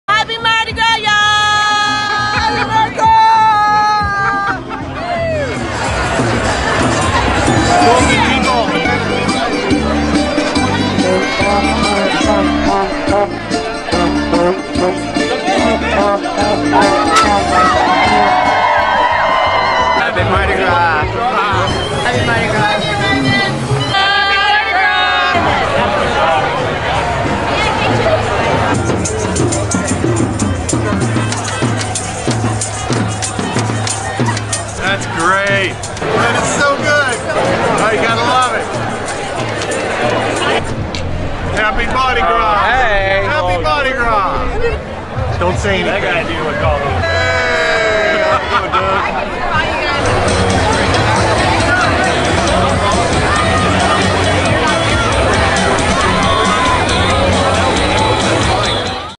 Mardi Gras 2017 in the Marigny
The intersection near Mimi’s bar in the Marigny neighborhood of New Orleans is always a scene on Mardi Gras morning, as costumers coalesce before heading off to the French Quarter. As always, the 2017 collection of hand-made costumes was one of the world’s great ad hoc collages, a collective artistic masterpiece.